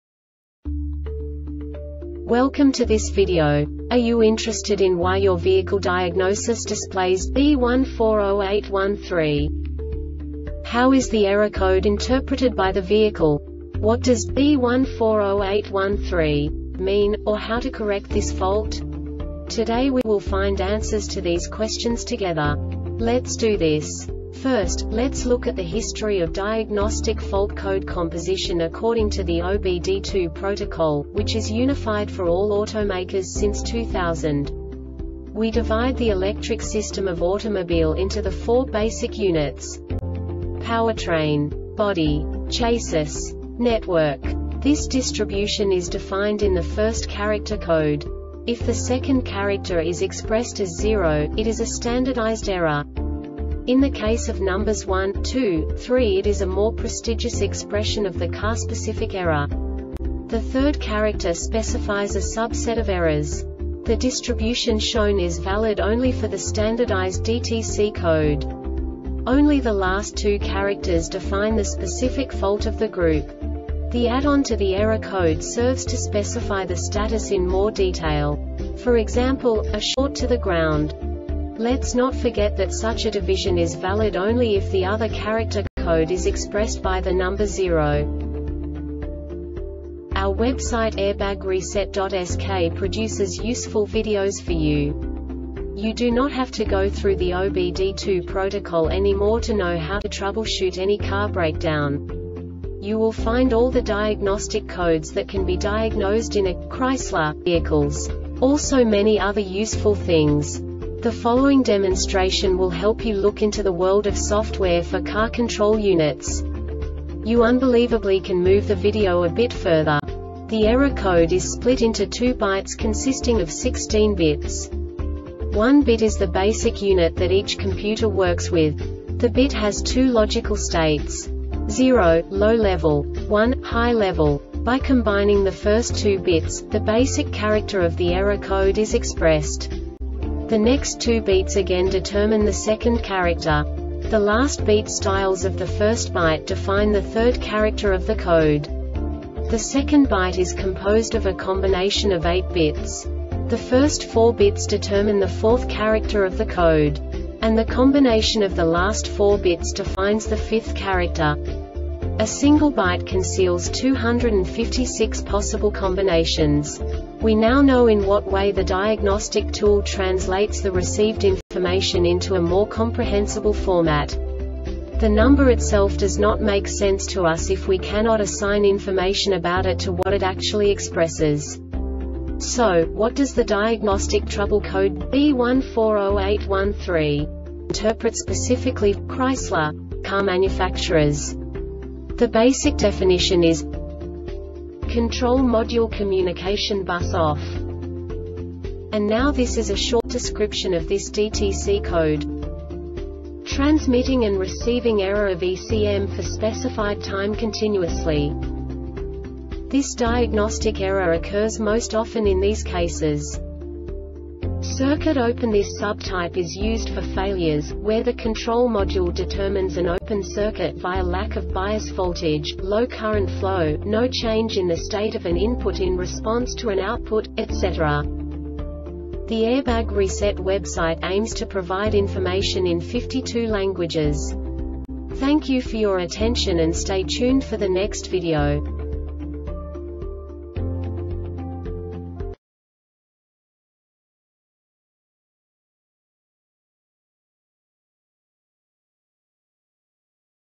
DTC Chrysler B1408-13 Short Explanation
Contents:
0:21 Basic DTC analysis according to OBD2 protocol standard.
1:48 Insight into programming
2:58 A diagnostically understandable description of the Diagnostic Trouble Code
3:05 Basic error definition

Make:
Chrysler
Code:
B1408 13
Definition:
Control Module Communication Bus Off
Description:
Transmitting and receiving error of ECM for specified time continuously.
Failure Type:
Circuit Open